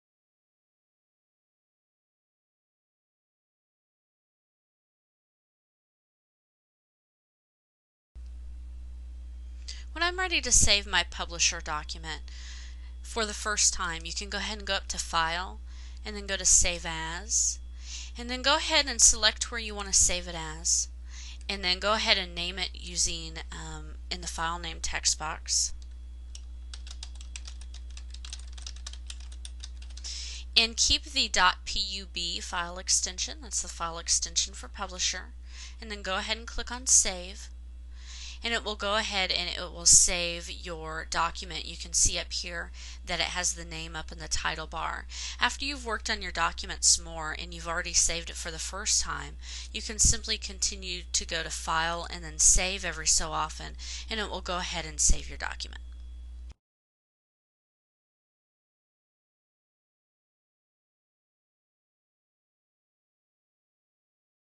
Save a Document in Publisher 2007
See a higher quality video on TechTutor.TV! The first time you save a Microsoft Publisher 2007 document, you can use the "Save As" command to name and save the document. After that, you can use the "Save" command to periodically save the document. In this video tutorial, you will learn how to save a document in Publisher 2007. See higher quality tutorials all for FREE